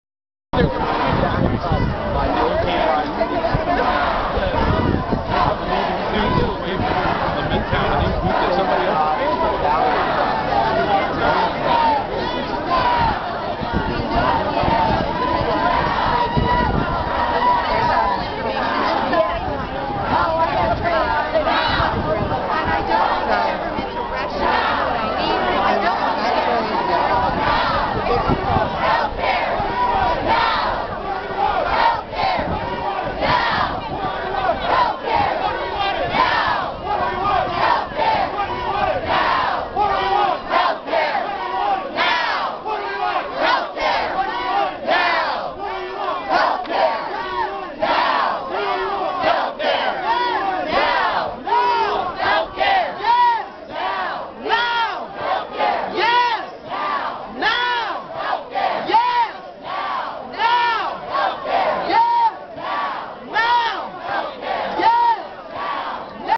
Anti-Obama care and Pro-Obama care demonstrators at UCLA - 8/21/09
Demonstrators at Henry Waxman's phony town hall meeting, UCLA - 8/21/09